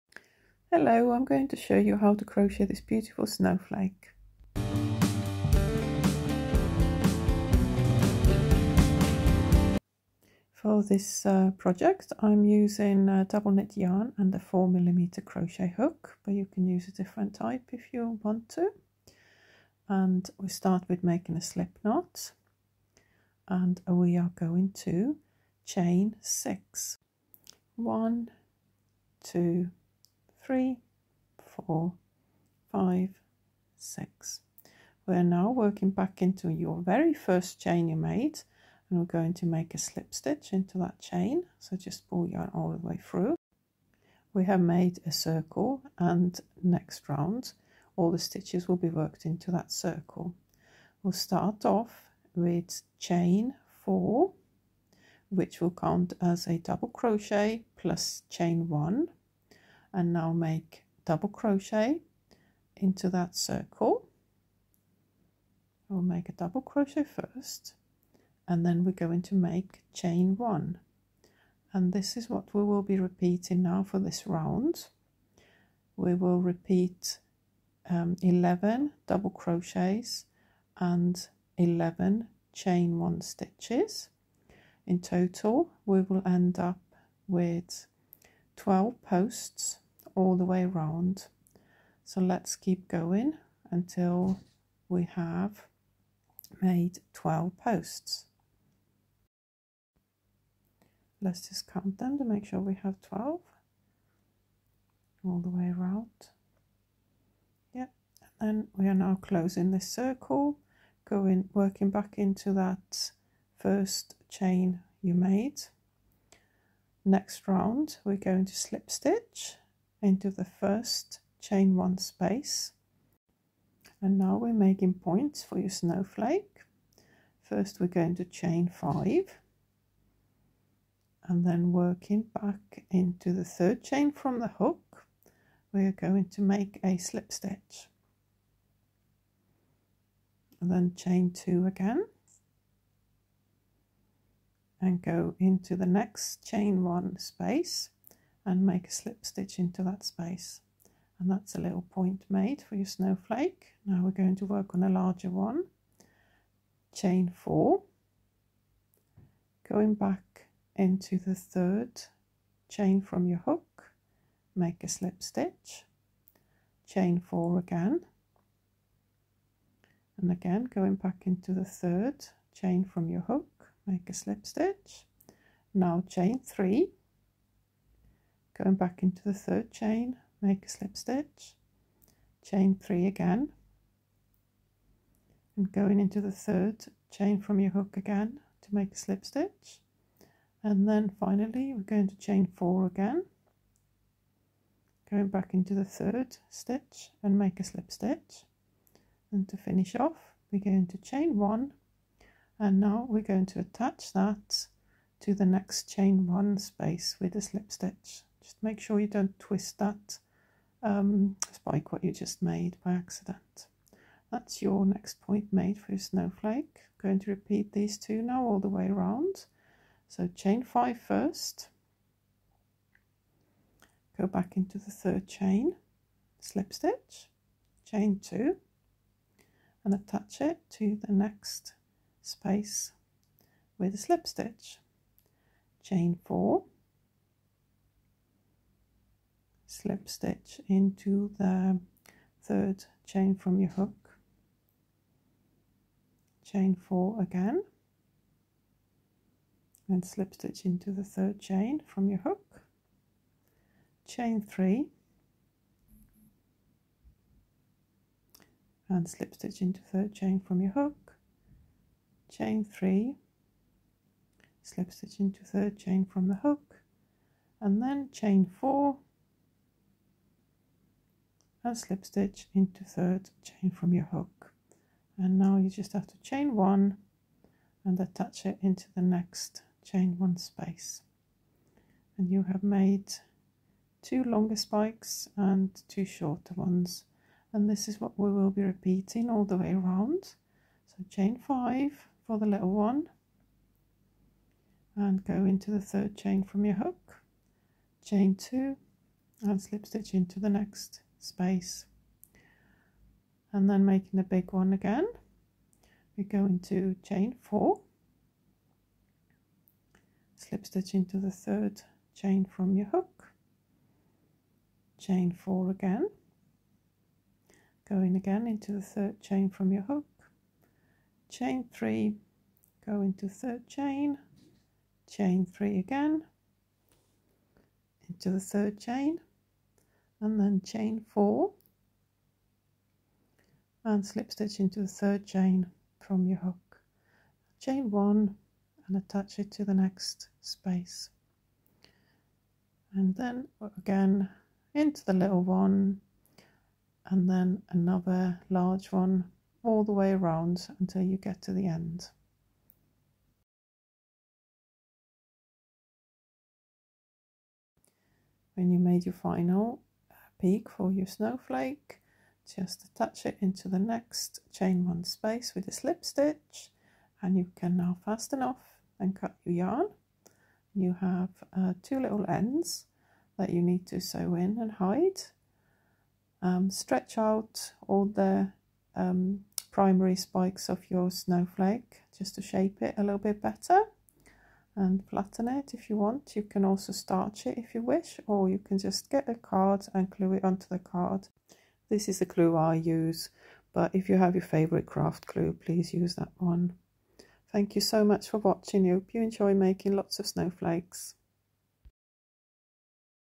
How to crochet a snowflake with just two rounds
Crochet tutorial using US Crochet terms